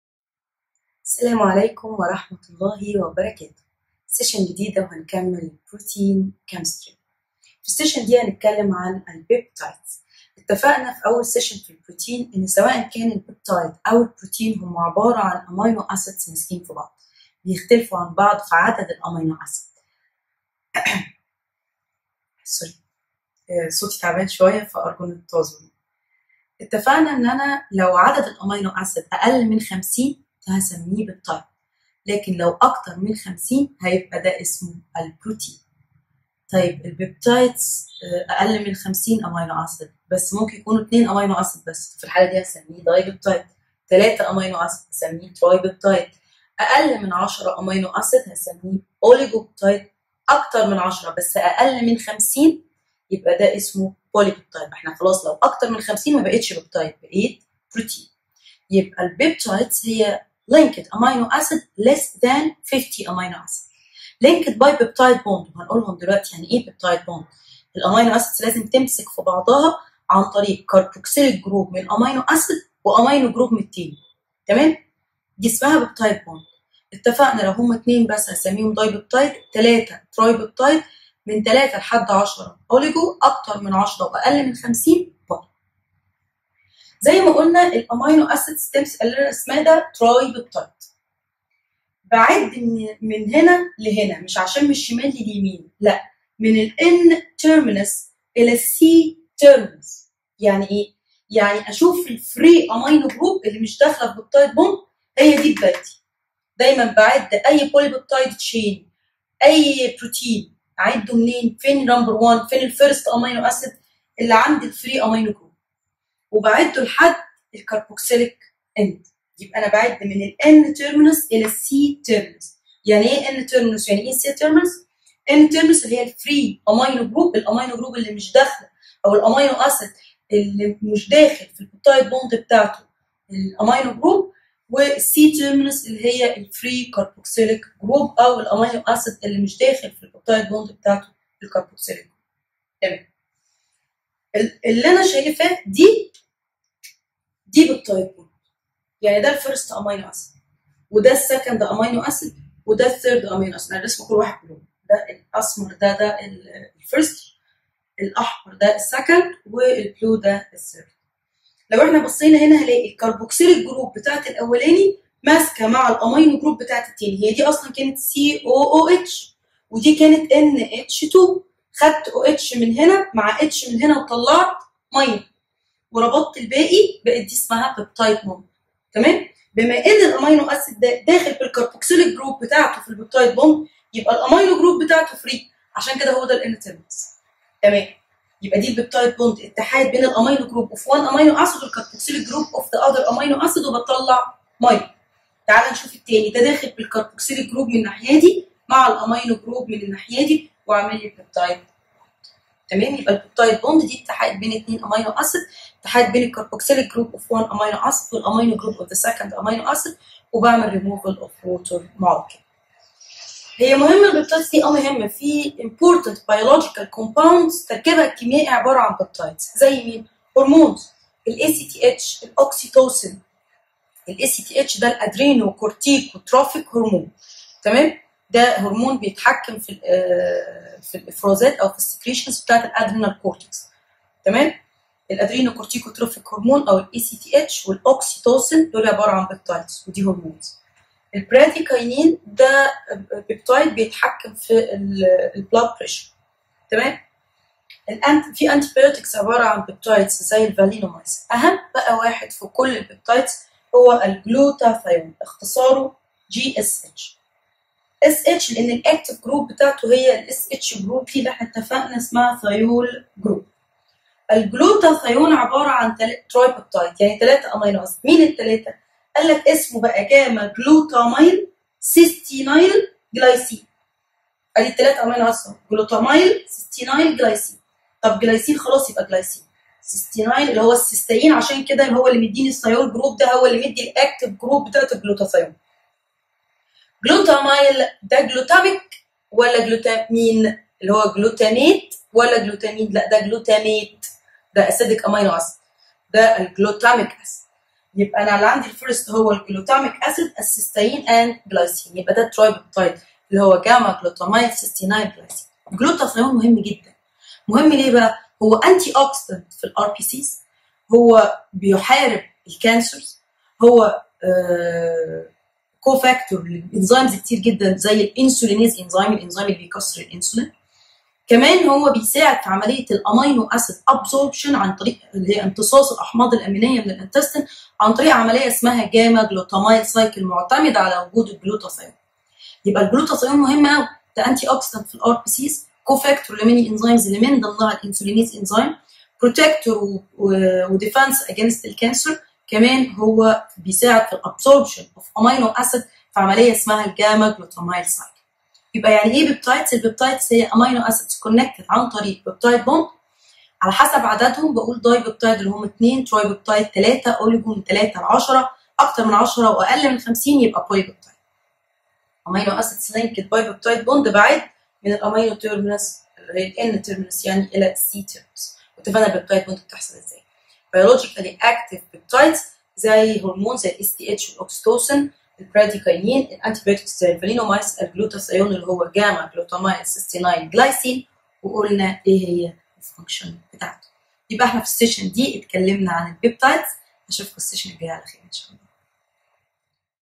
(Protein Chemistry Session 4)Peptides
Definition of Peptides
types of peptides
Biologically Active Peptides
Glutathione